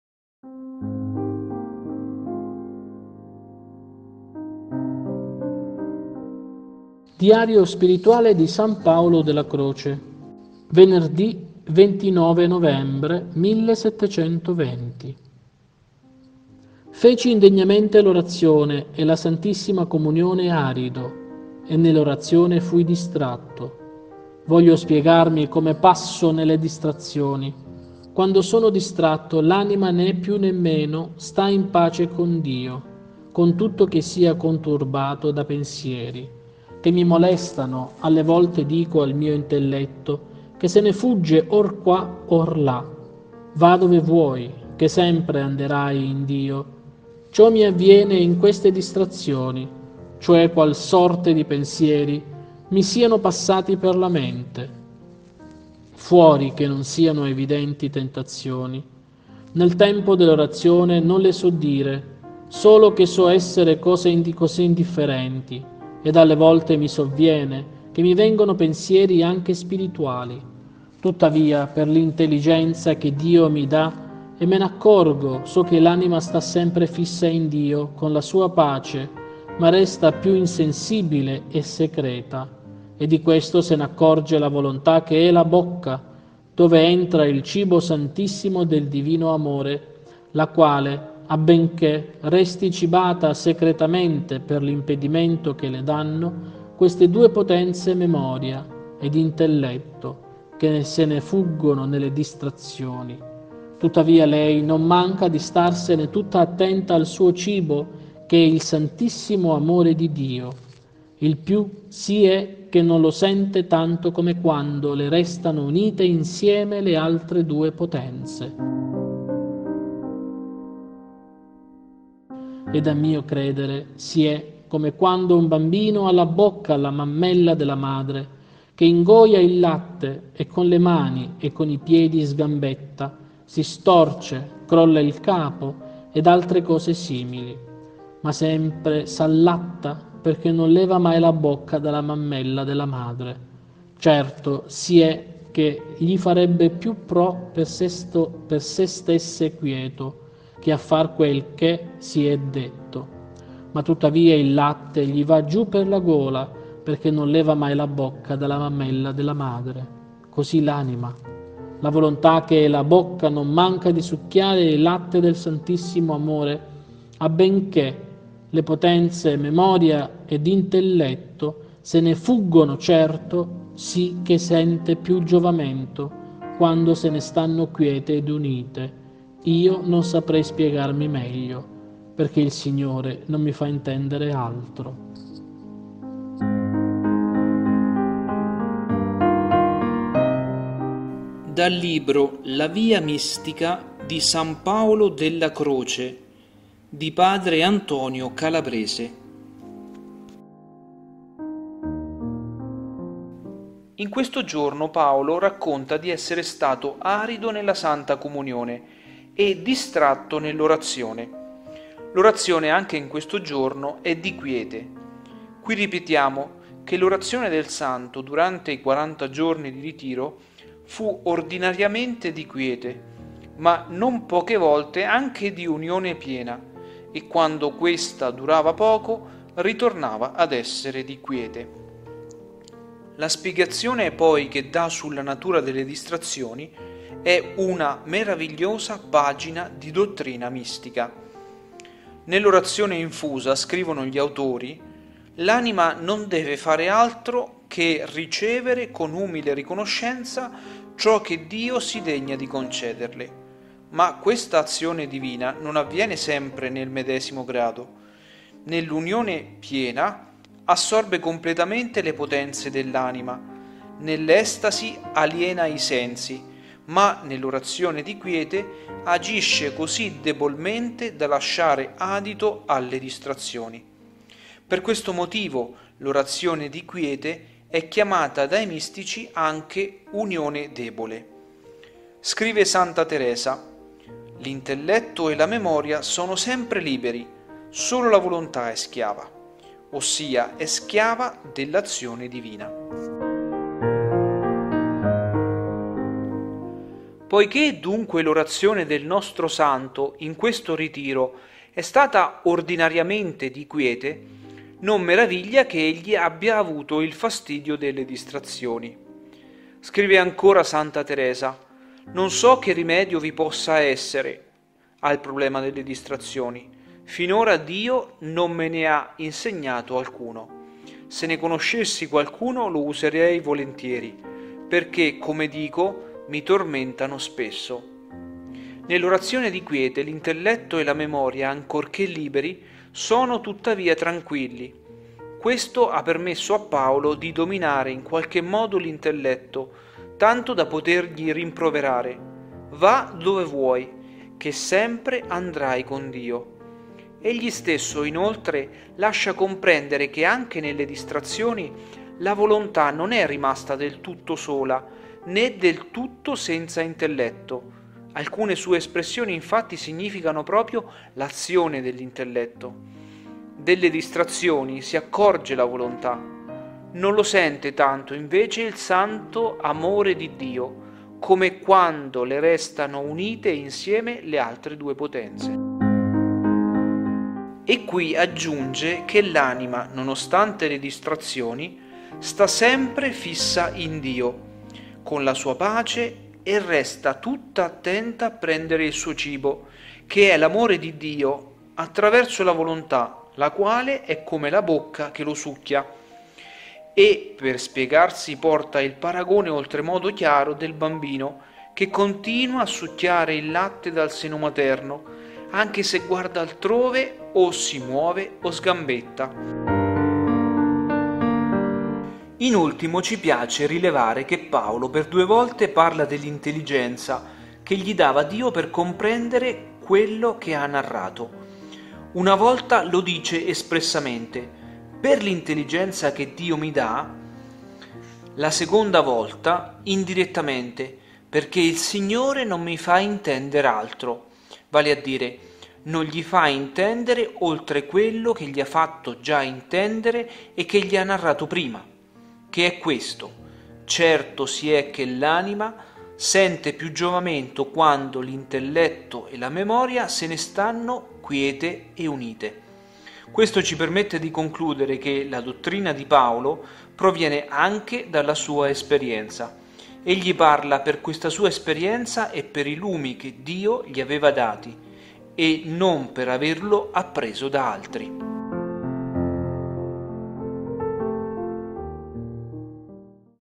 Diario Spirituale 29 Novembre
Diario Spirituale di San Paolo della Croce
Lettura del Diario: LoZio Salvo
Lettura del Commento: Marco Staffolani
A cura dei passionisti della Scala Santa.